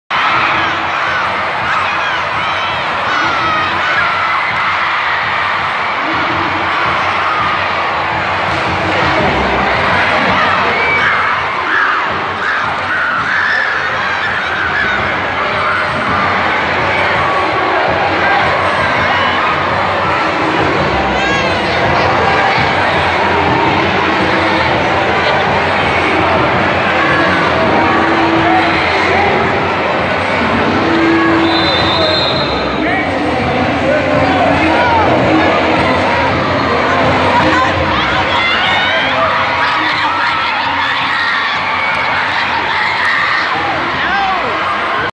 Middle School pep rally dance battle!
A Lancaster Middle School pep rally dance battle